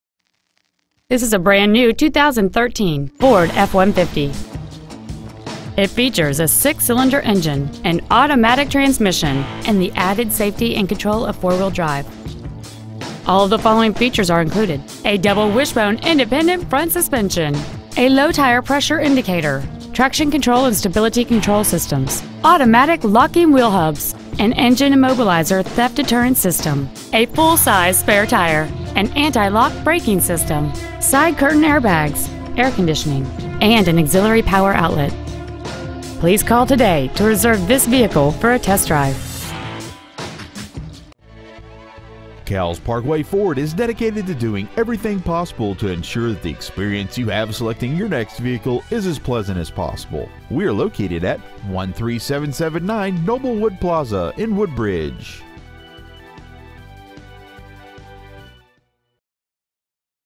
2013 FORD F-150 Woodbridge VA
Year : 2013
 Make : FORD
 Model : F-150
 Engine : 3.5L V6 24V GDI DOHC TWIN TURBO
 Trans . : 6-SPEED AUTOMATIC
 Exterior : RUBY RED METALLIC
 Miles : 10
 Interior : PALE ADOBE
 Stock : 130901